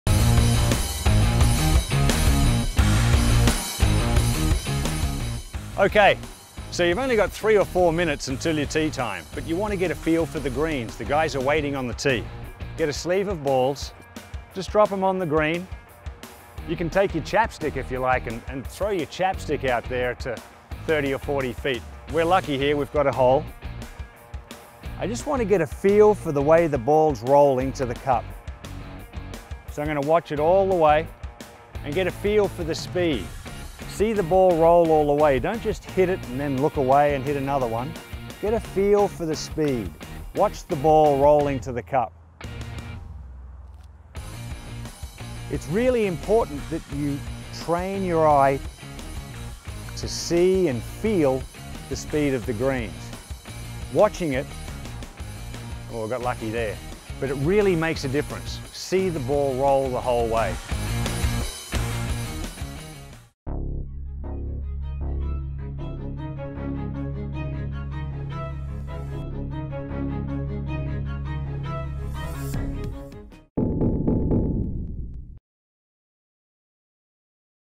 Quick Putting Tips: Feel The Greens With Ian Baker-Finch
What's that? Your tee time is in 5 minutes but you want to get an effective putting warm up in? Use this drill from Ian Baker-Finch.

Having an issue with a certain part of your putter game?